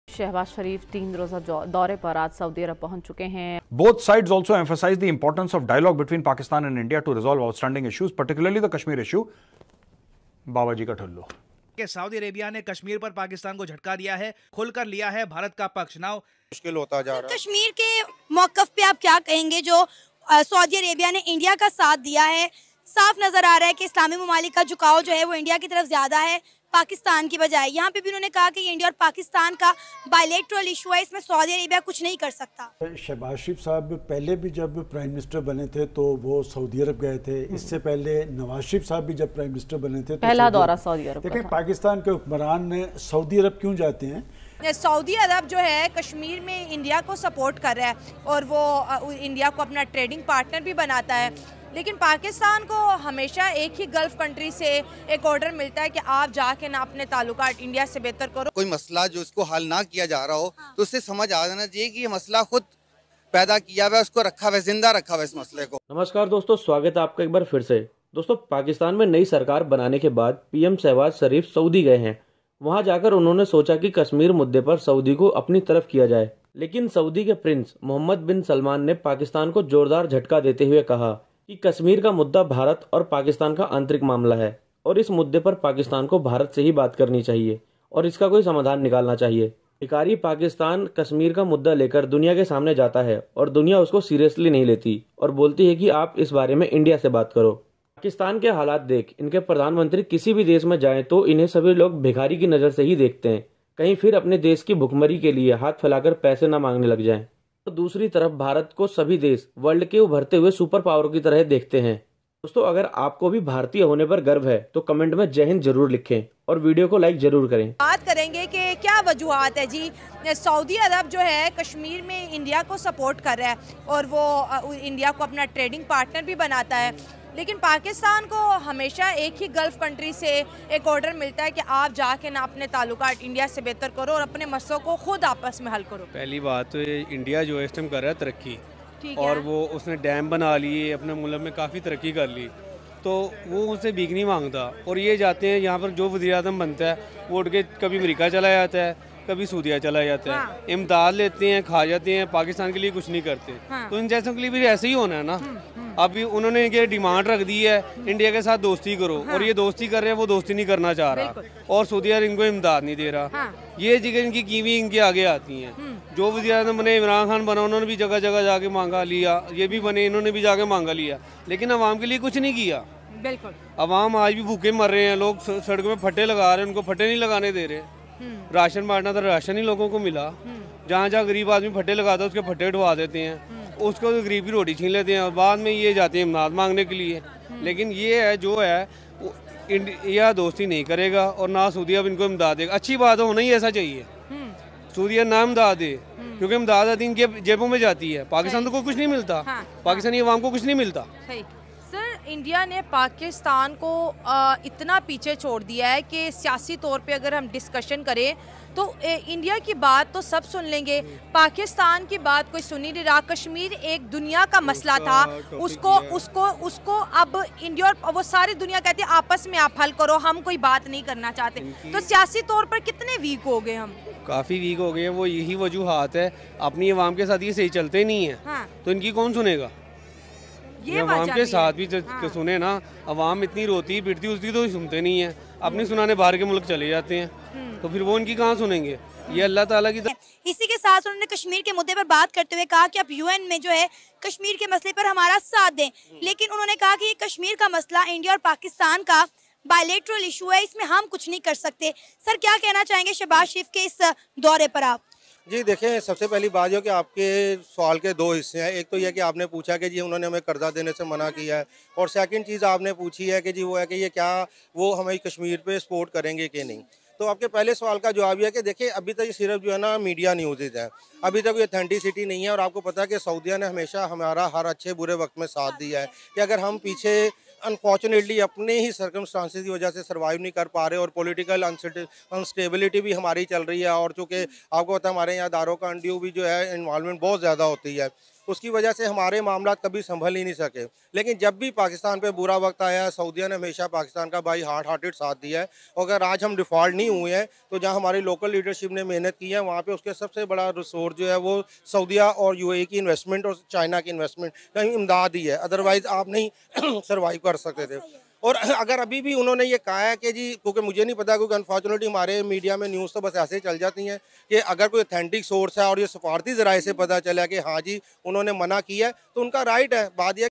USA-Saudi Arab gave a setback to Pakistan over Kashmir Issue | hindustan special
पाकिस्तान को दिखाई औकात USA-Saudi Arab gave a setback to Pakistan over Kashmir Issue | hindustan special

this is the official channel of "hindustani reporter"